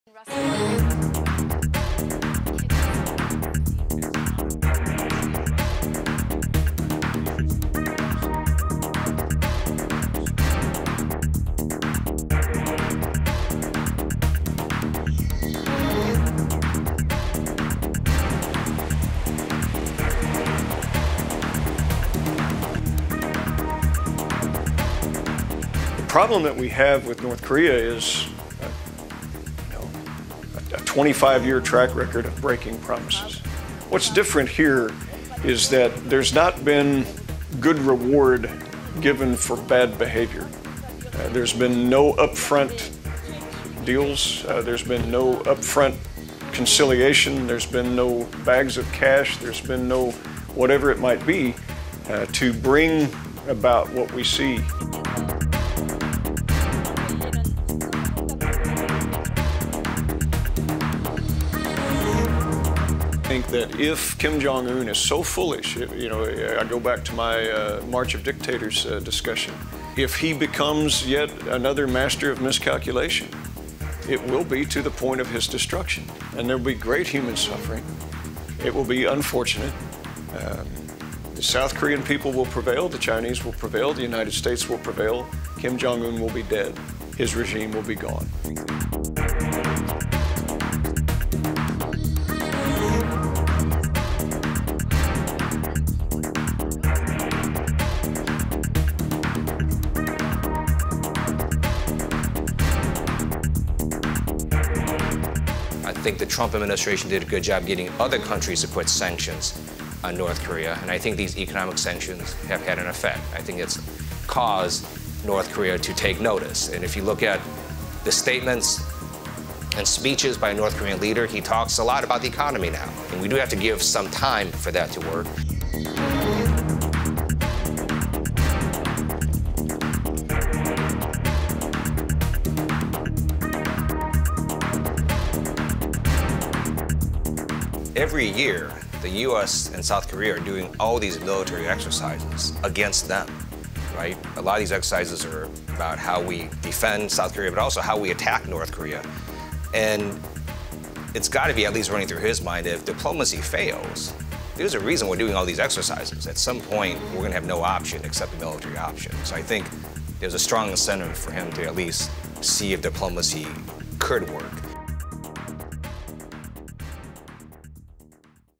“김정은 어리석은 판단하면 결과는 재앙”
스티브 러셀 미 공화당 하원의원이 미국평화연구소 간담회를 통해 미국은 한반도의 평화, 경제적 이익을 위해 북한과의 협상에 나서고 있다고 말했습니다. 북한은 지난 25년간 약속을 깬 전력이 있지만 북한의 나쁜 행동에 미국이 좋은 보상을 하지 않았다는 점에서 이번 협상은 다를 것이라면서 북한 김정은 위원장이 어리석은 판단을 한다면 결과는 재앙이 될 것이라고 경고했습니다. (취재: 조상진 / 영상편집: 김정호)